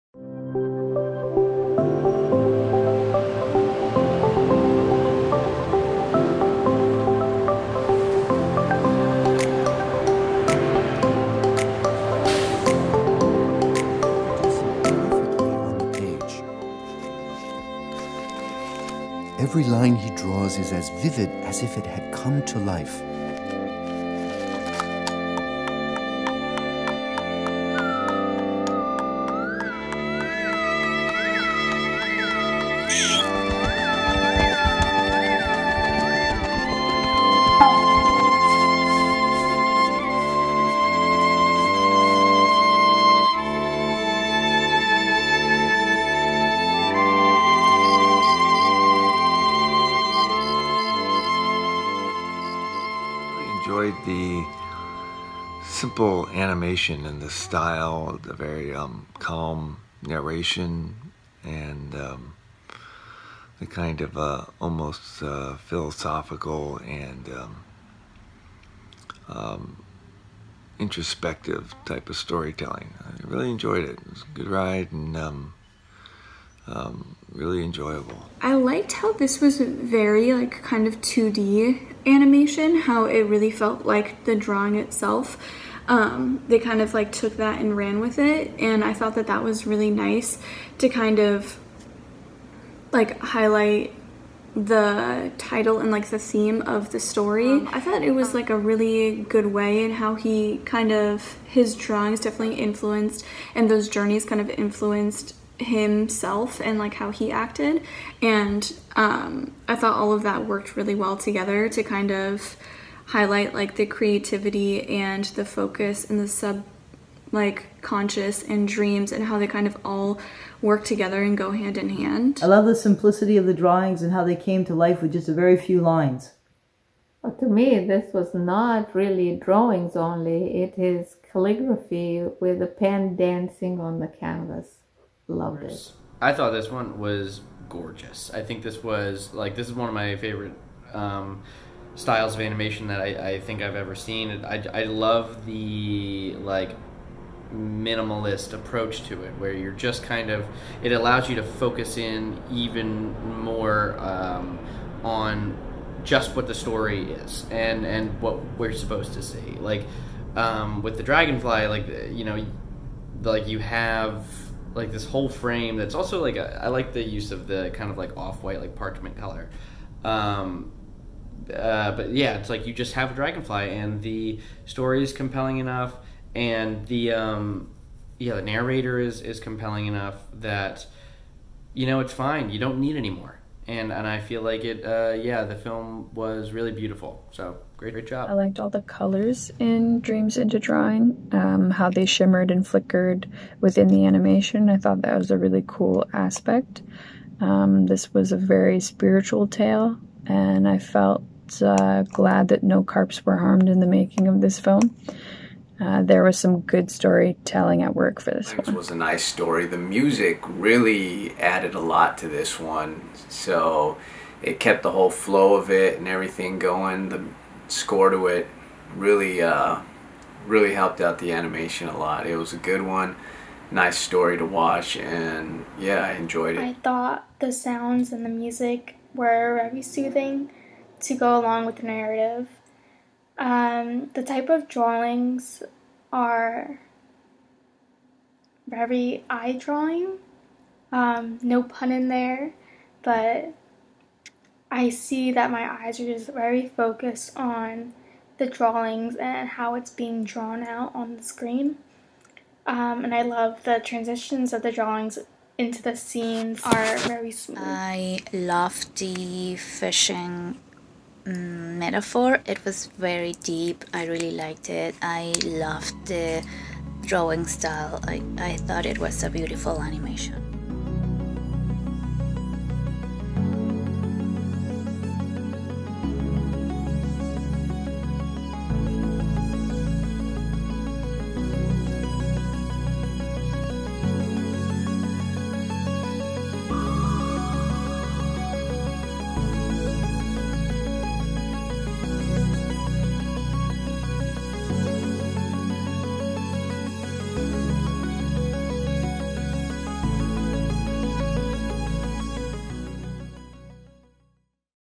Dreams Into Drawing Short Film, Audience FEEDBACK from June 2020 ANIMATION Film Festival
DREAMS INTO DRAWING, 10min., Japan, Animation
Directed by Koji Yamamura

The painter KUWAGATA KEISAI lived over 200 years ago. He illustrated Ways to Sketch a series of picture books depicting humans and animals, all richly expressed. One day he was sketching a carp when suddenly he fell asleep and, in his dream, transfigured into a carp. The carp was enjoying his swim, but when a fisherman caught him things suddenly took a turn for the worse…